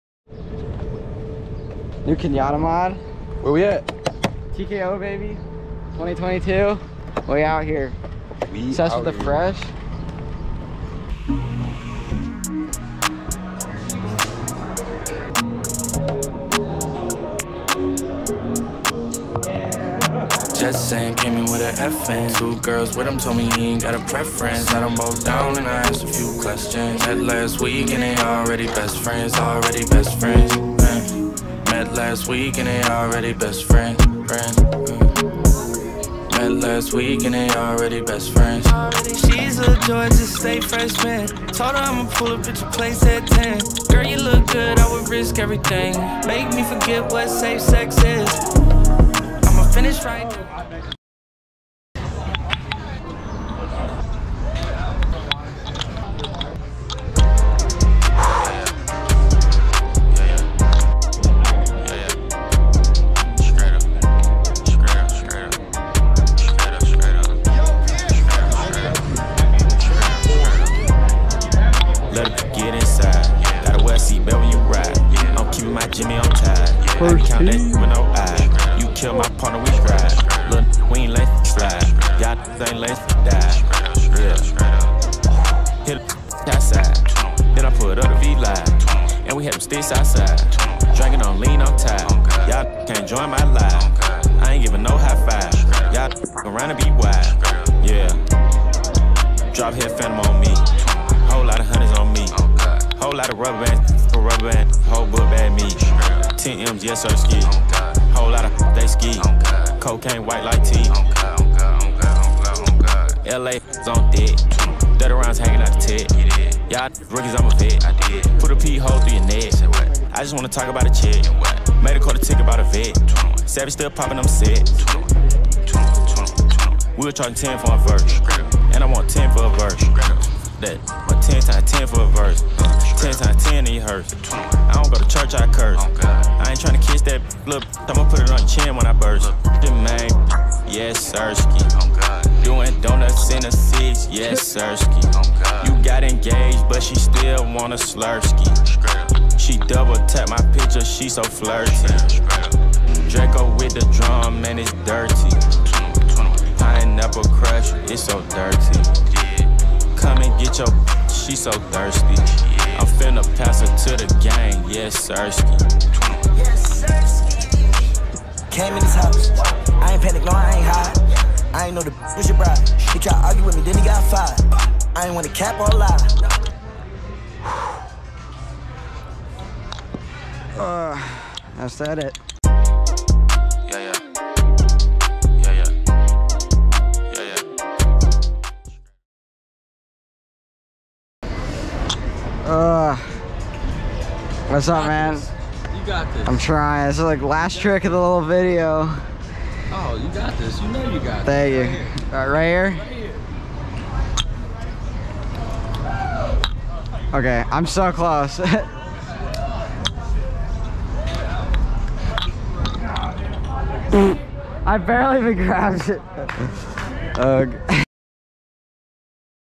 Kendama USA - Benny Boy VS The Trill Mod!
During TKO we gave Ben the all new Kenyatta Williams Pro Model and a Handed him a GoPro. To say the least the boy went to work... Tune In and watch Ben Pop off on all new The Trill Mod!

Shot/Cut - Benny Boy
Dama Used - Kenyatta Williams Pro Mod
Location - Chattanooga, TN

The Trill Mod is Available Now!